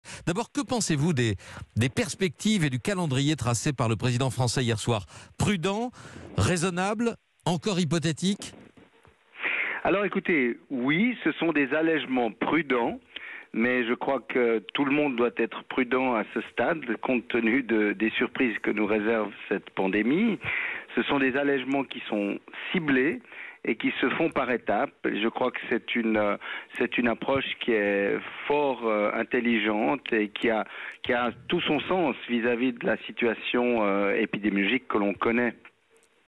Covid-19 : pourquoi la barre des 5.000 contaminations par jour est-elle si importante ?
Les annonces d'allègement du confinement, annoncées par Emmanuel Macron, mardi soir, respectent un calendrier très précis et toujours le même objectif : faire baisser les contaminations à 5.000 par jour. Le médecin épidémiologiste, spécialiste de l'hygiène des mains et du contrôle des infections, Didier Pittet, était l'invité de Patrick Cohen sur Europe 1.